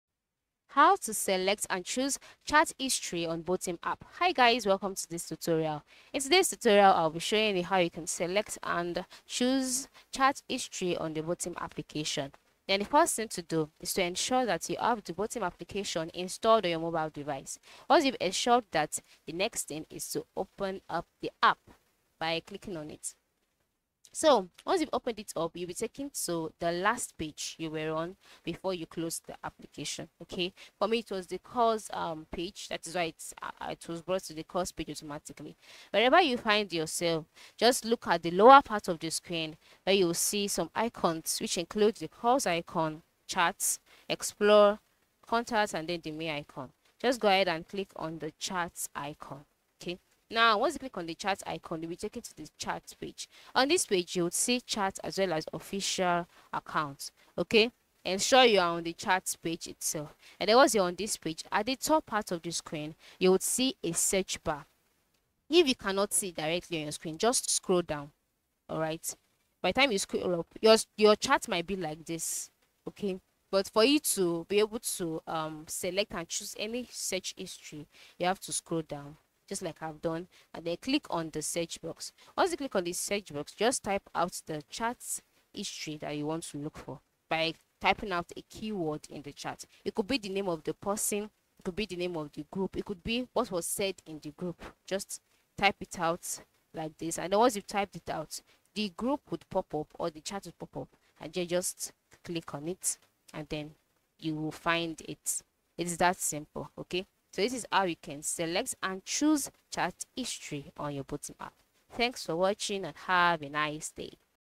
How to Select and Choose Chat History on Botim App (Easiest Way)
Today we talk about How to Select and Choose Chat History on Botim App , so stay until the end of the video to see the full explanation.